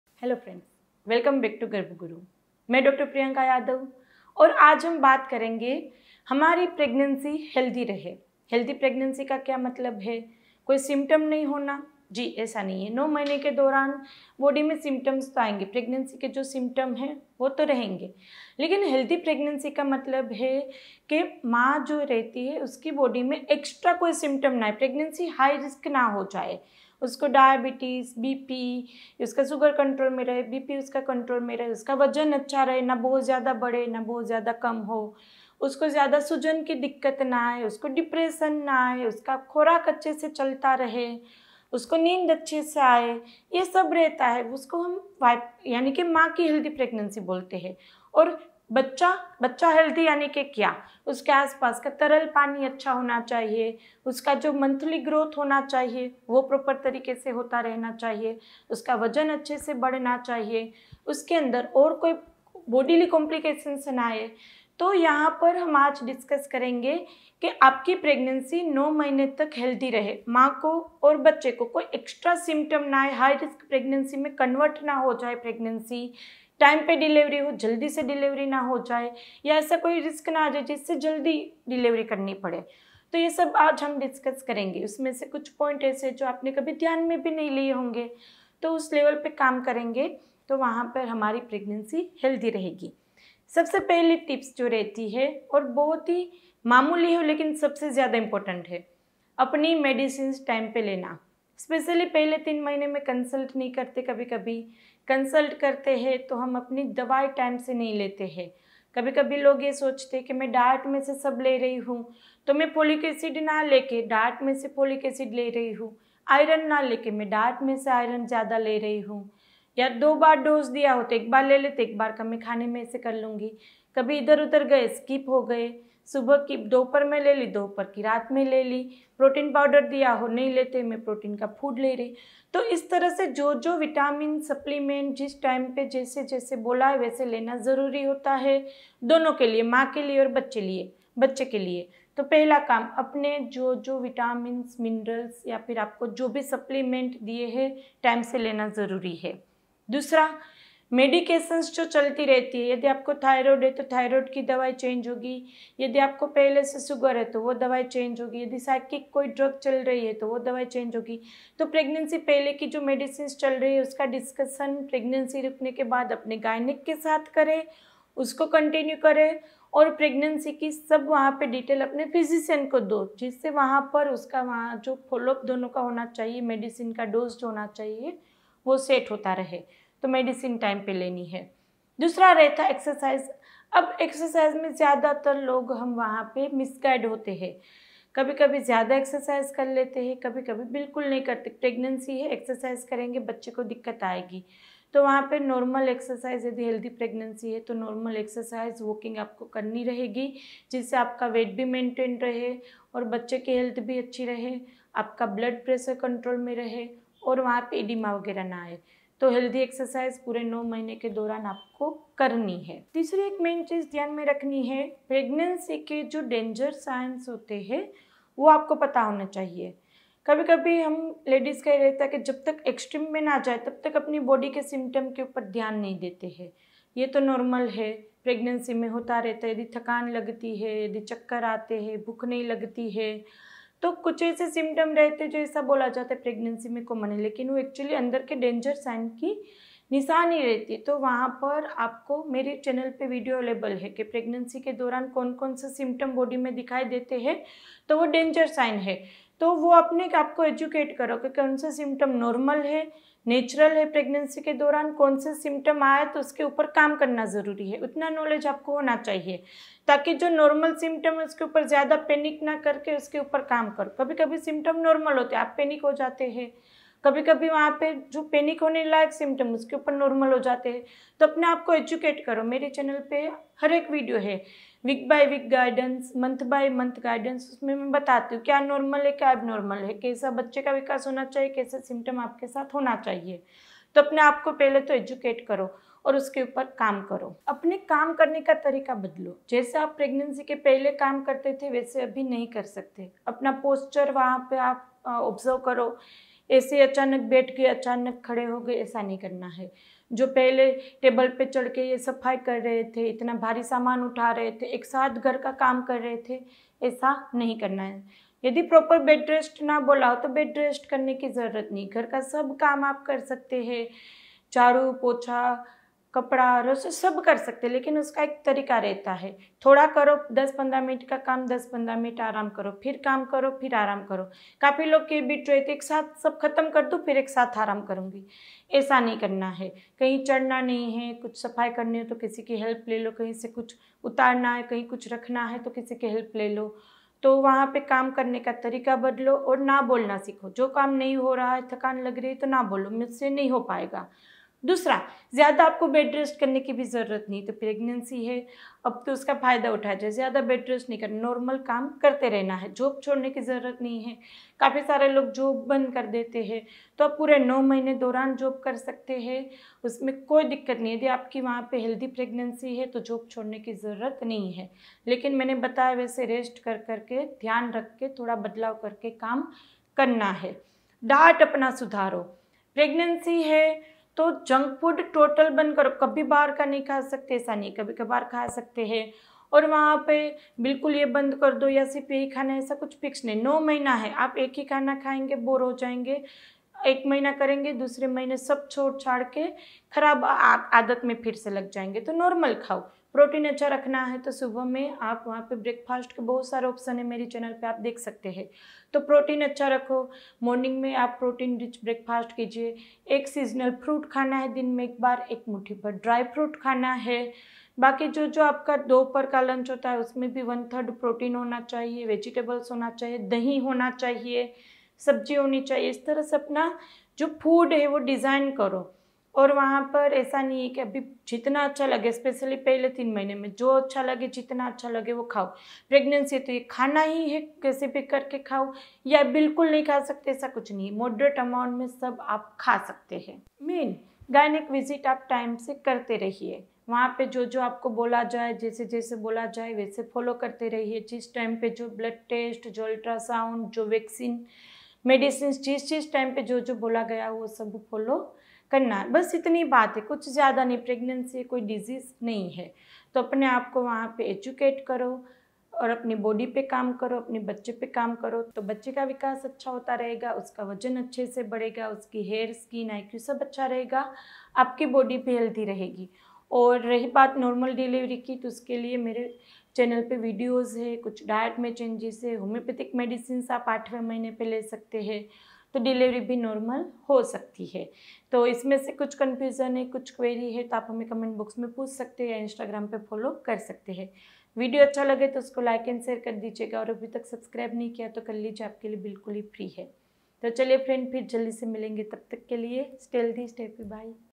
सुबह में ये 3 चीज़े खालो, बच्चा गोरा, सुंदर और मजबूत होगा | TOP 3 TIPS FOR HEALTHY PREGNANCY
Is video me maine pregnancy me baby ke achche vikas ke liye morning diet me kya kya food khana chahiye, kaunsi exercise karni chahiye aur kaunsi tips follow karni chahiye uske baare me baat ki hai.

                              ***Online Store***
Urine Pregnancy Test Kit
Baby Products